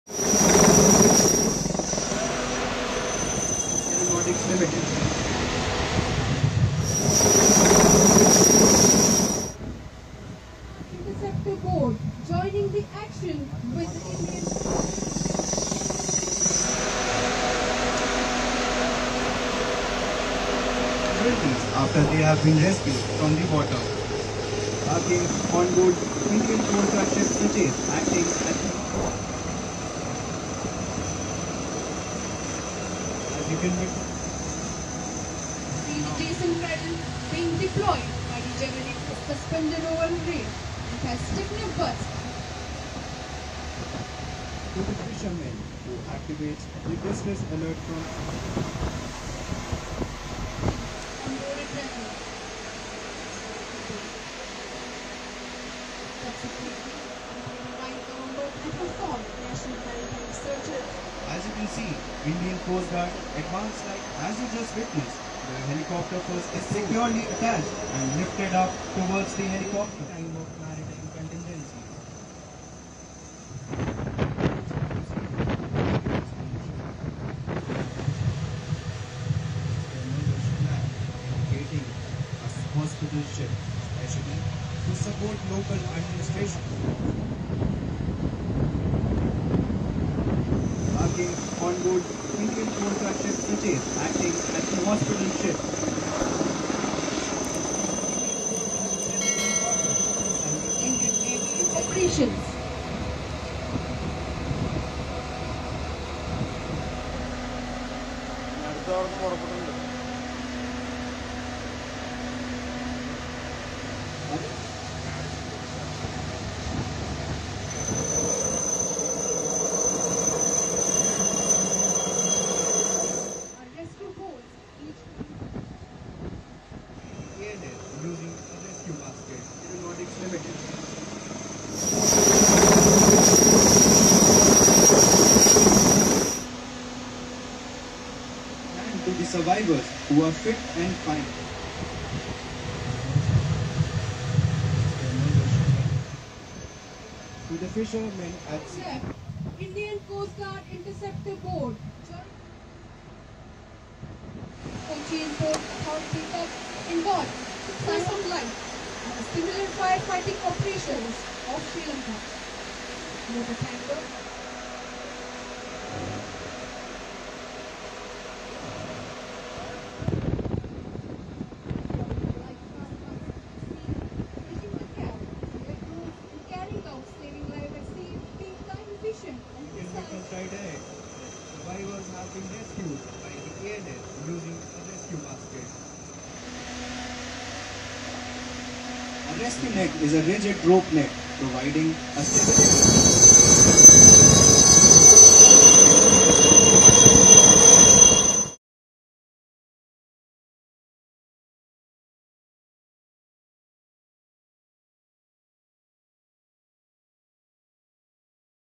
From sharp maneuvers to rescue op drills, Indian Coast Guard showcases its prowess off Kochi coast
In multilevel exercise, Indian Coast Guard on November 29 carried out a sea exercise, SAREX -2024. The ICG conducted 11th edition of the National Maritime Search and Rescue Exercise off the Kochi coast. During sea exercise, a coordinated mass rescue operation was swiftly demonstrated with deployment of several resources. The exercise witnessed a scenario portraying a passenger aircraft that was stranded in the waters and passengers were rescued from it. Ships and aircraft from Indian Coast Guard, Indian Air Forces, tugs from Cochin port, Kochi water metro and water ambulances took part in the event. SAREX-2024 was held on the theme ‘Enhancing Search and Rescue Capabilities Through Regional Collaboration’. Through SAREX-2024 Indian Coast Guard aim at strengthening maritime safety, rescue operation capabilities.